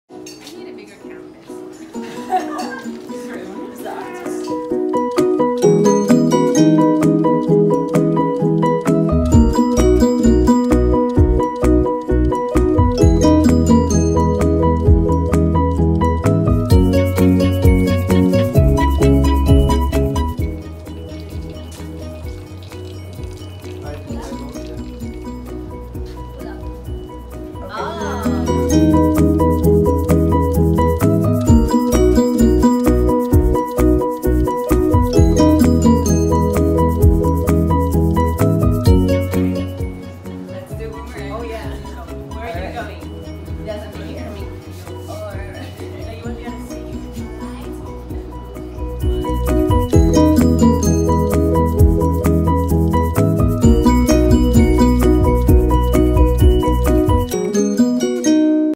British American Tobacco (Dunhill) Batik Team Building
Wonderful afternoon with all the talents from BAT ( British American Tobacco) Asia Pacific team , for the Batik Team
Building